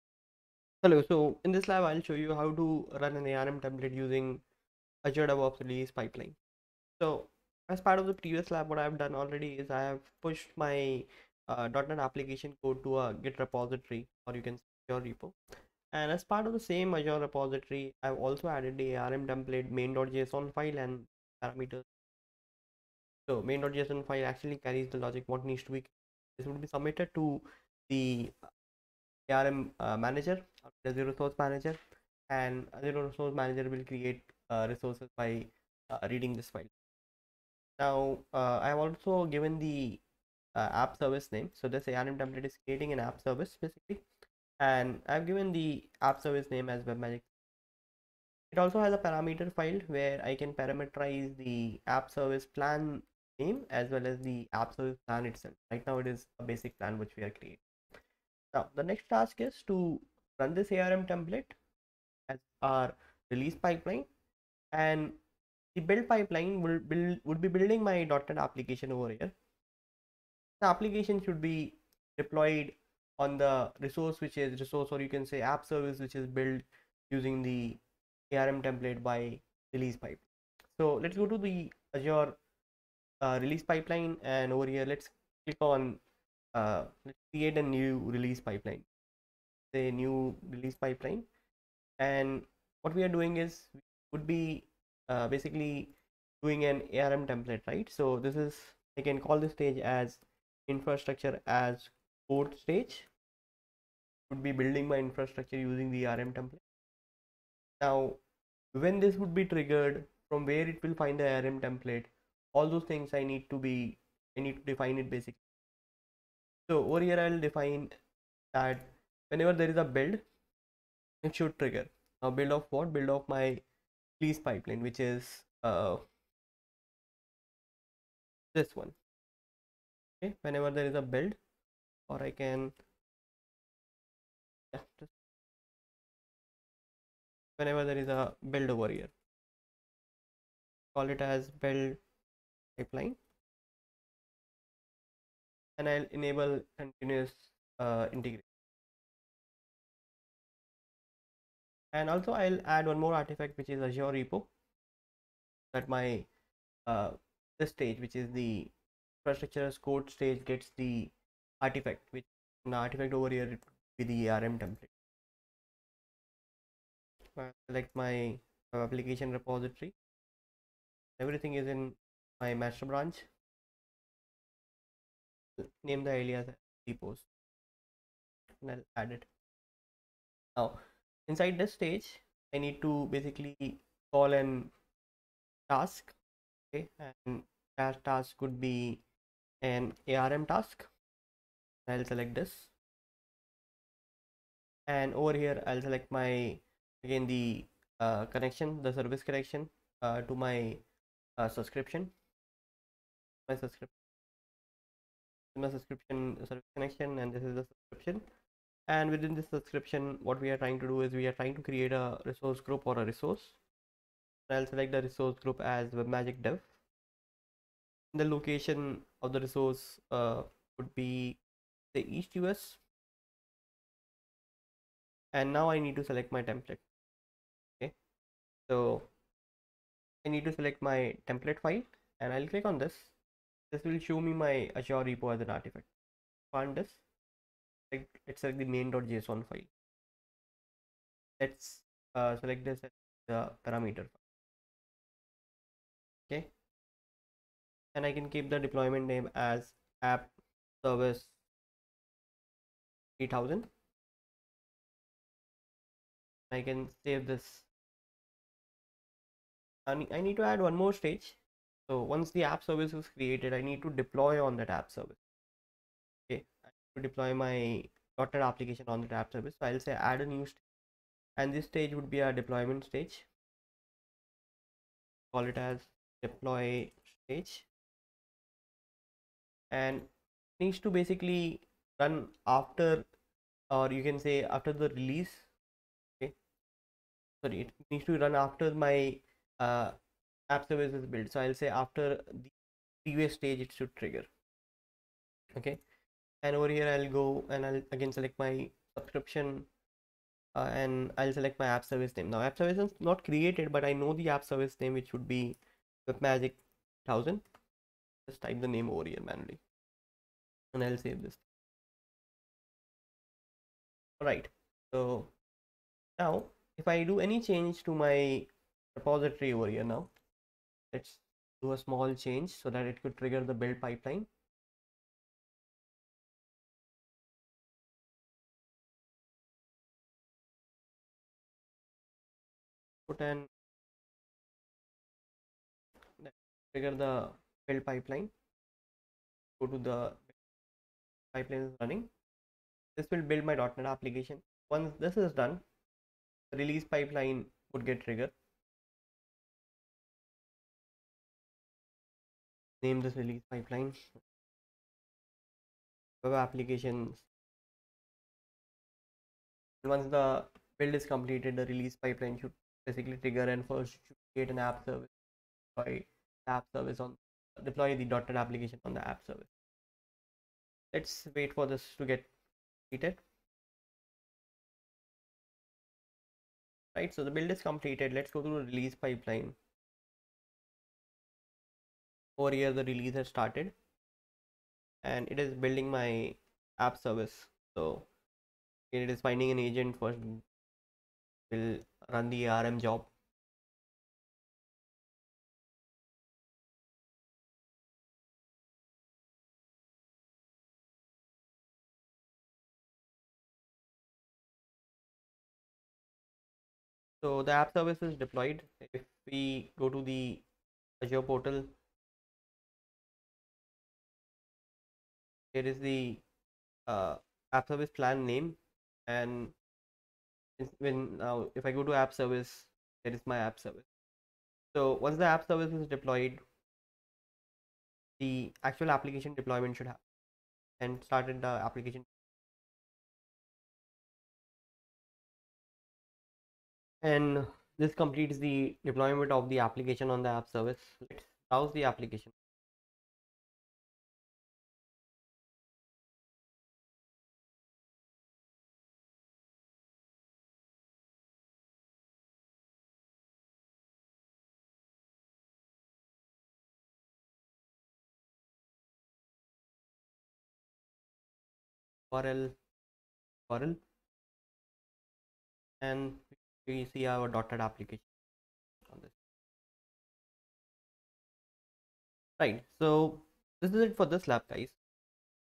Running ARM Template Using Azure Release Pipeline
This lab shows you how to Run ARM Template Using Azure Release Pipeline